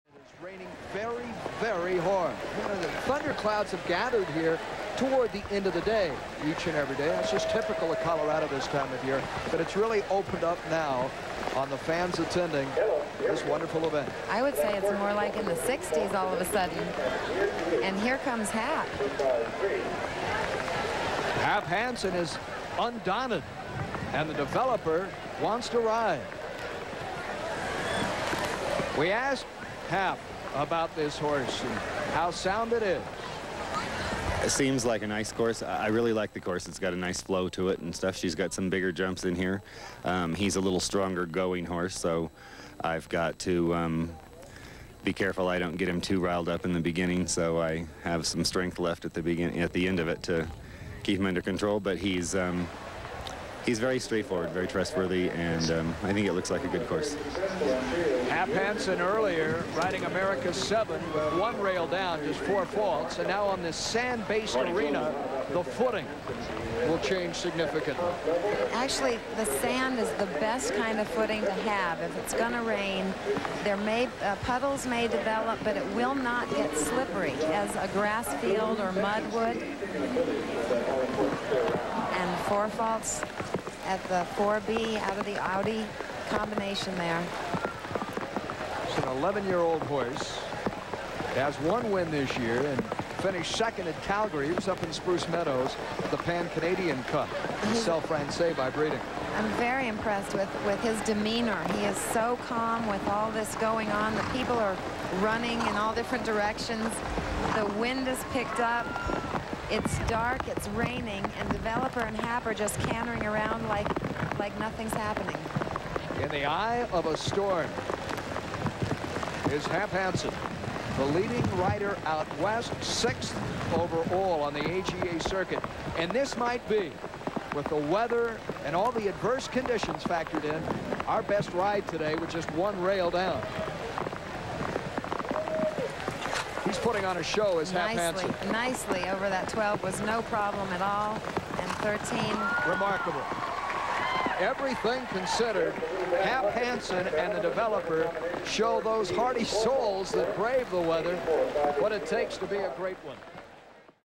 SHOWJUMPING: Hap Hansen - Developer in a Deluge
The Developer... what a cool horse!

Mean as a snake, but an amazing athlete by any regard-- just look at those knees over fences!

Here he's seen in the 1997 Grand Prix of Denver, at the beginning of a torrential thunderstorm.  The horse demonstrates amazing grace under fire, as he and hap put in a great round in classic fashion!

This class was won by Damian Gardner (RIP) on Pinon Lily, with the winning round viewable here: